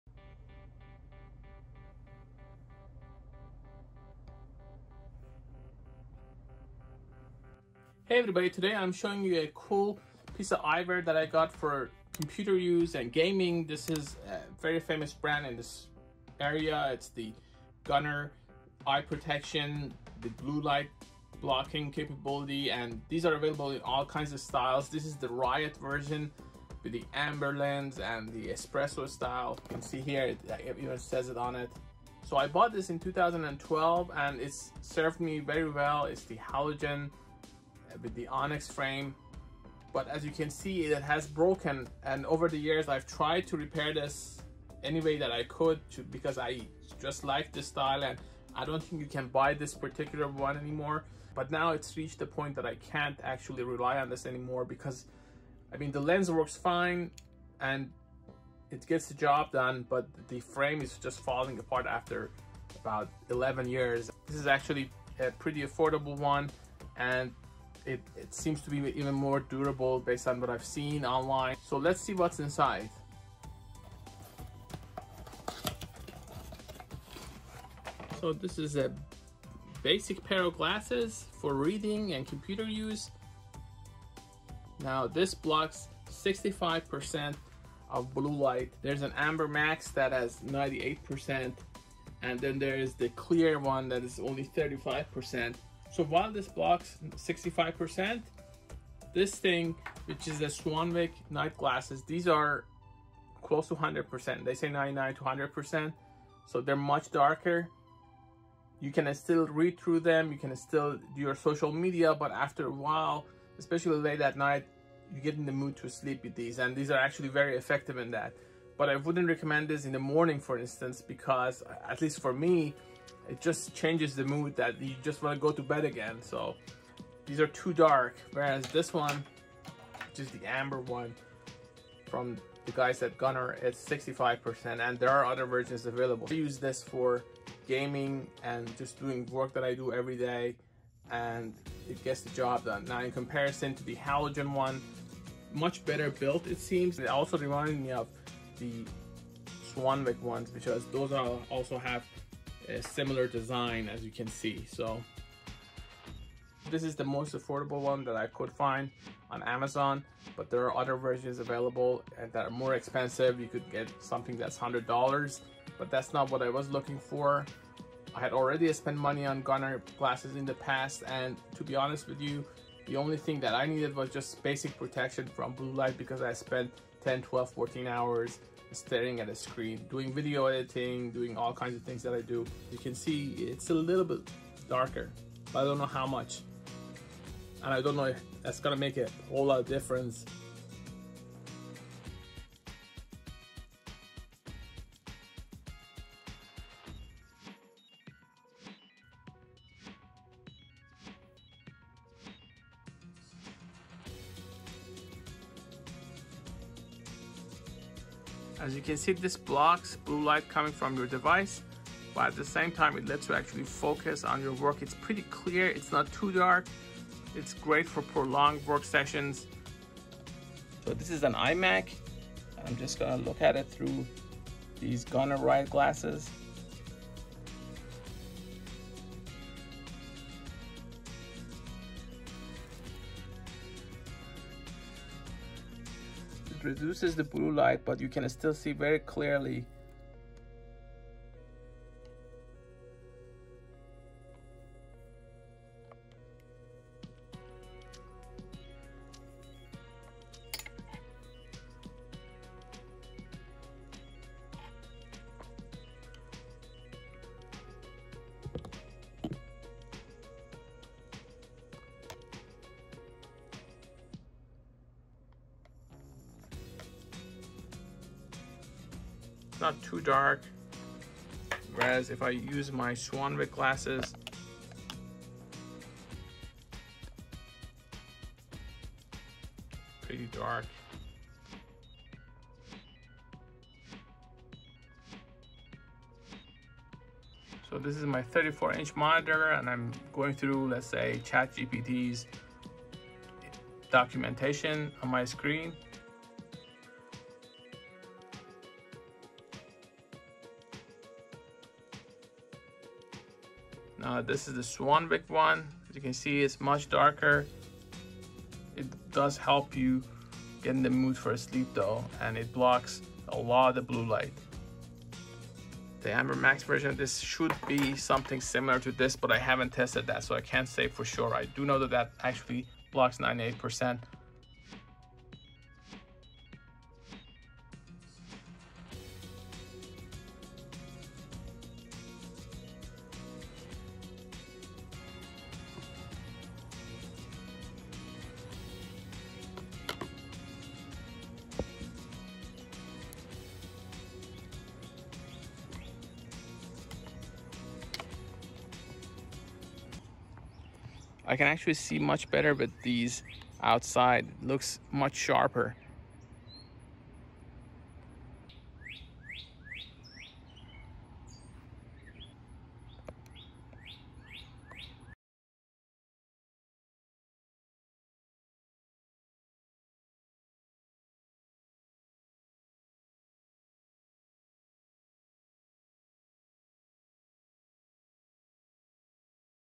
GUNNAR Espresso Riot Computer Gaming Glasses Tested 🟡 Gadgetify
👉 Where to get GUNNAR Espresso Riot Glasses (*aff)

These are GUNNAR Espresso Riot Computer Glasses: gaming glasses that block 65% of blue light to go easier on your eyes. These reduce glare and enhance focus. They are great to use indoors and outside. In this video, I put these glasses to the test and also compare them to Halogen and Swanwick glass.